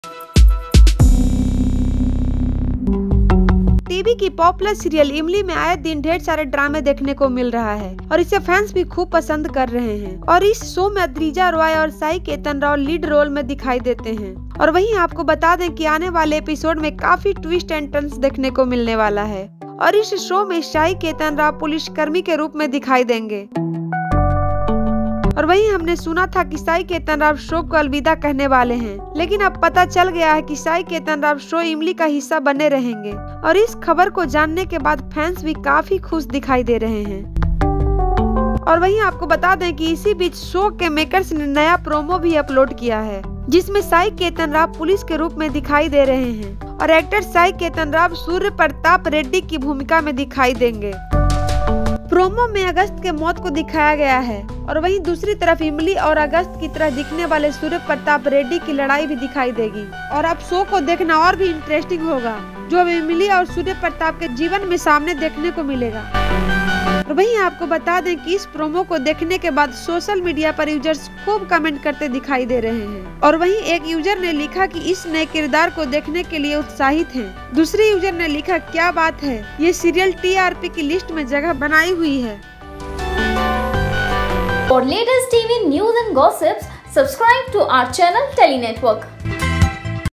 Imlie 3 में Sai Ketan Rao पुलिस ऑफिसर किरदार से मचाएंगे धूम, Agastya के नए खुलासे पर कहानी में मोड़
Imlie 3 में Sai Ketan Rao पुलिस ऑफिसर के किरदार से मचाएंगे धूम, Agastya के नए खुलासे पर कहानी में मोड़ ।